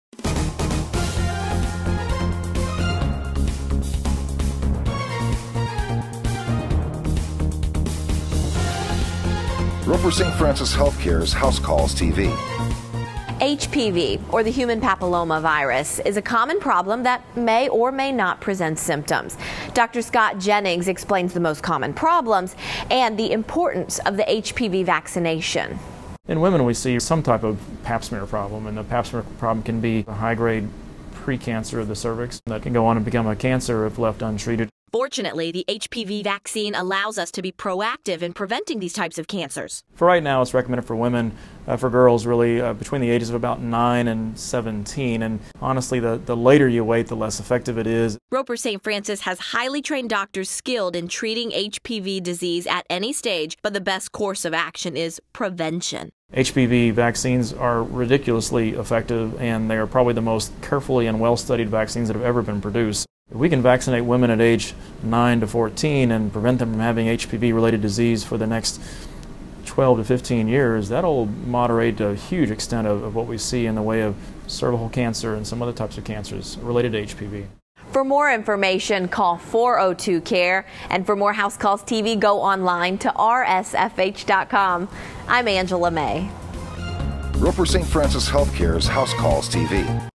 Human Papilloma Virus (HPV)
Dr. Scott Jennings explains the most common problems and the importance of the human papilloma virus (HPV) vaccine.